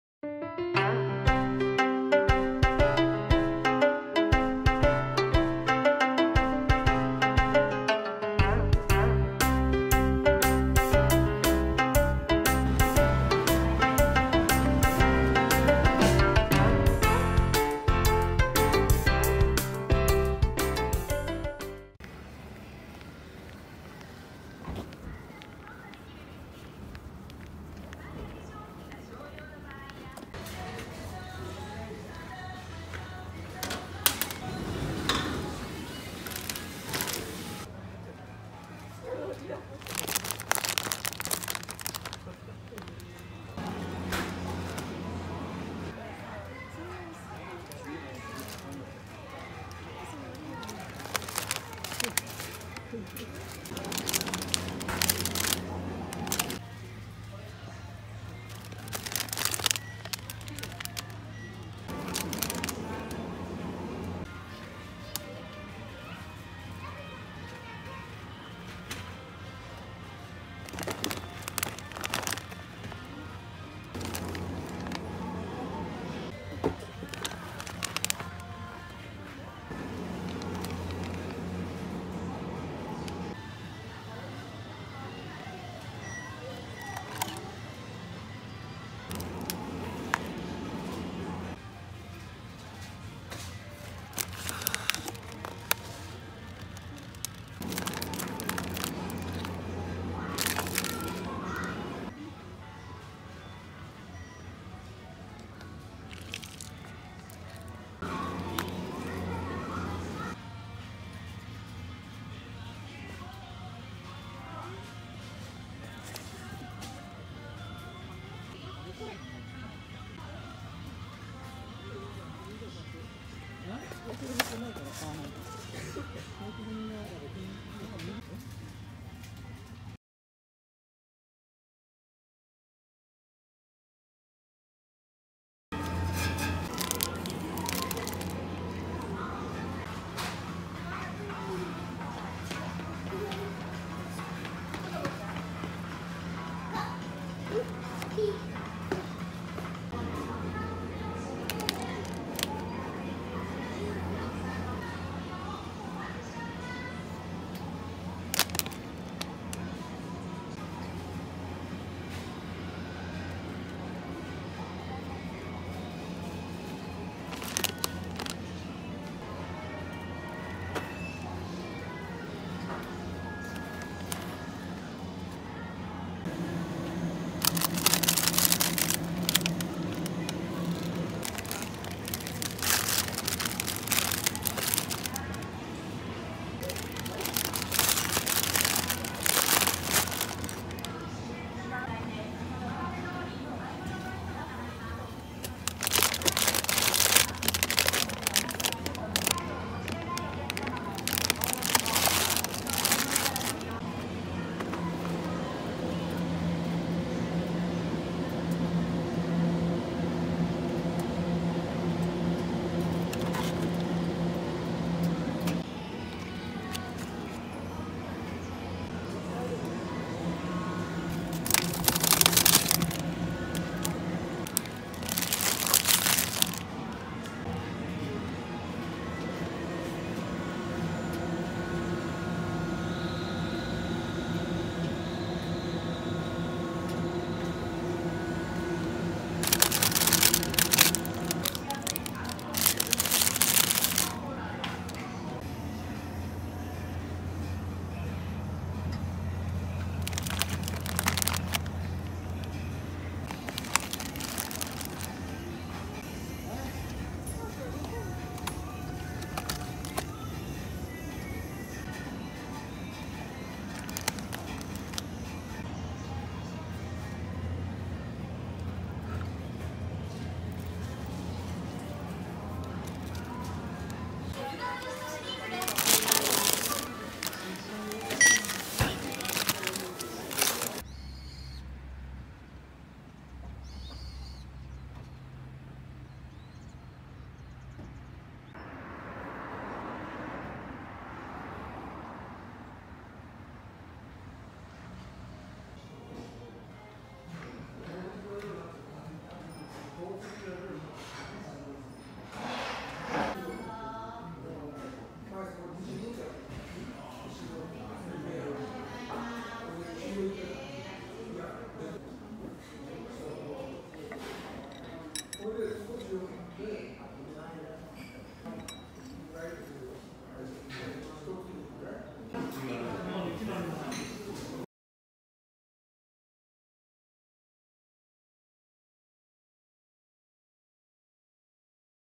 Grocery Shopping in Japan / a cup of coffee and some bread for free
Why don’t you try the “morning service “ in Aichi, Japan?

If you order a cup of coffee then you can have some bread for free.
Bread with butter, jam or boiled egg!